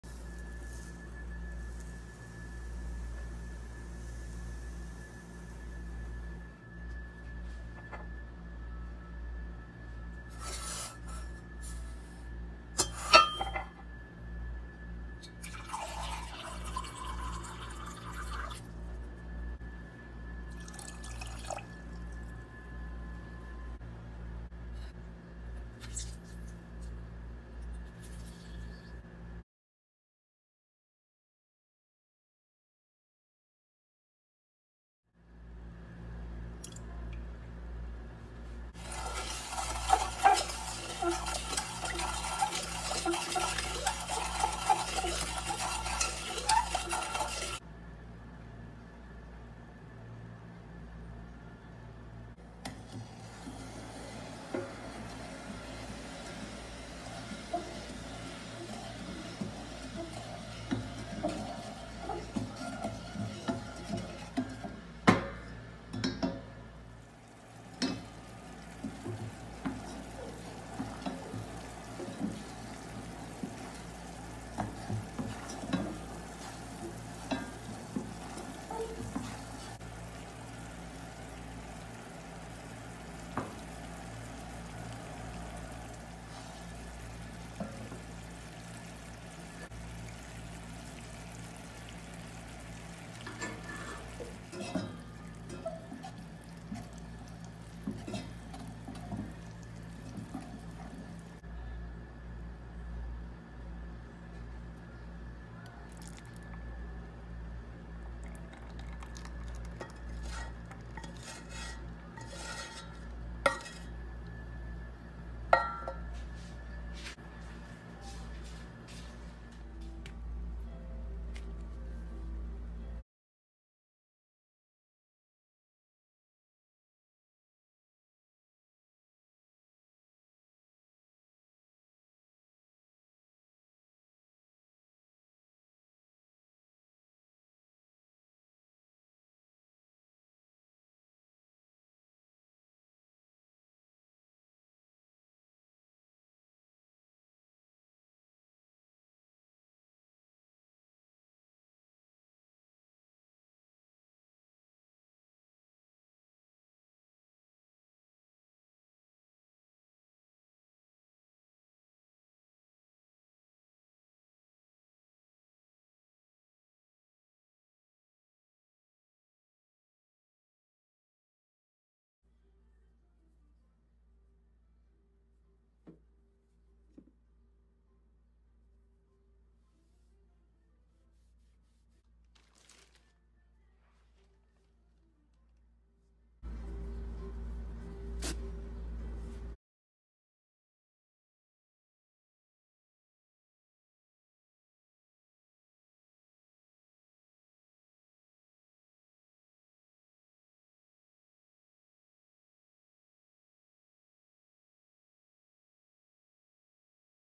أسهل طريقة لتحضير الحلاوة او السويت لازالة الشعرمن جميع مناطق الجسم بدون ألم 💯
طريقة عمل الحلاوة بملعقة واحدة من الليمون او بدون ليمون
المقادير
200 غرام من السكر
مايعادل 14 ملعقة كبيرة من السكر
6 ملاعق من الماء او
100 مل من الماء
ملعقة كبيرة من الليمون الحامض يمكن تعويض الليمون بالبرتقال المندرين او جريب فروت